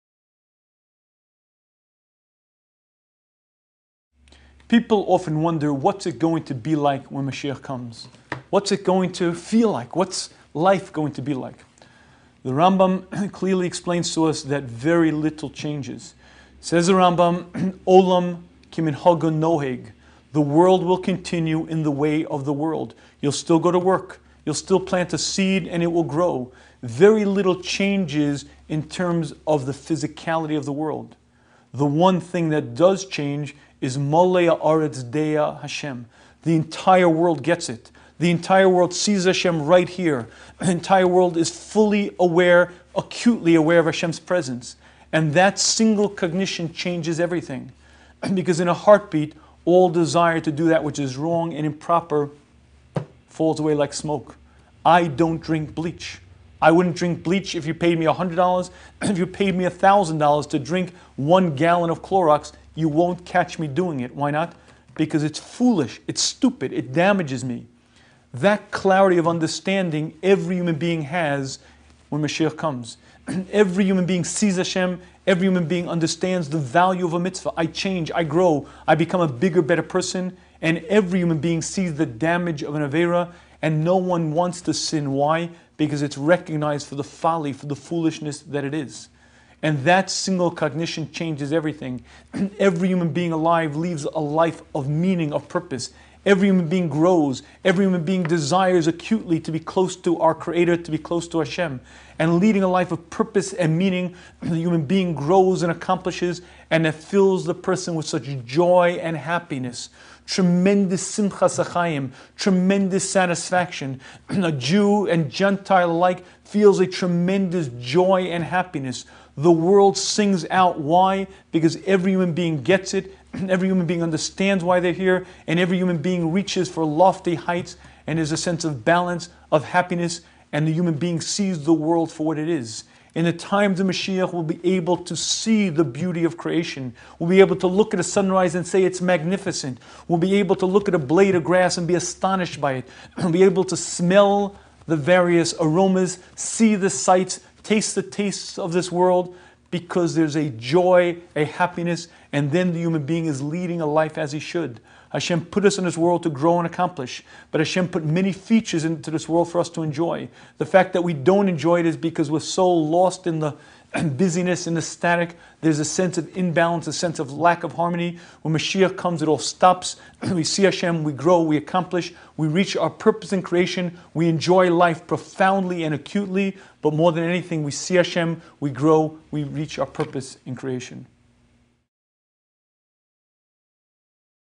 The Three Weeks: When Moshiach Comes
What will it be like when Moshiach comes?: Hear Rabbi Shafier explain single cognition that will change the world